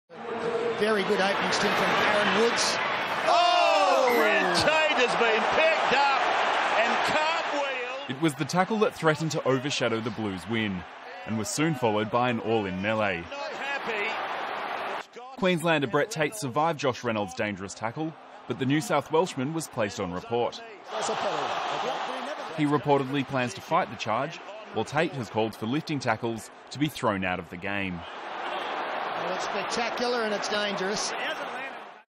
State of Origin 2014 Game one - Josh Reynolds dangerous tackle
NSW five-eighth Josh Reynolds has vowed to fight a dangerous throw charge on Brent Tate that is set to rub him out of game two, dumbfounded by the match review committee's ruling.